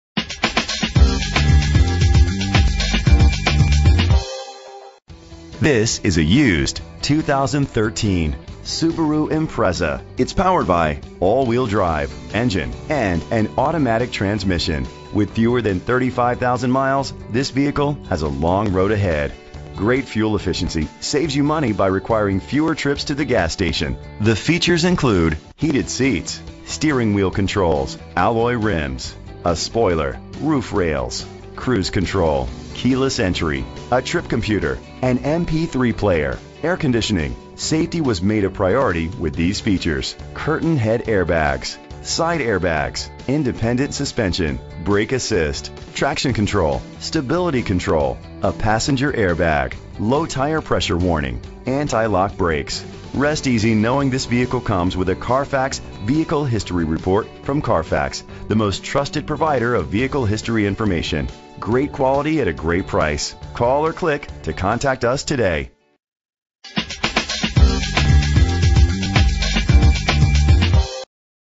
2013 Subaru Impreza - Bedford OH
Year: 2013
Make: Subaru
Model: Impreza
Trim: Sport
Engine: 2.0 L, 4 cylinder
Transmission:
Color: Red
Mileage: 33120
Address:
250 Broadway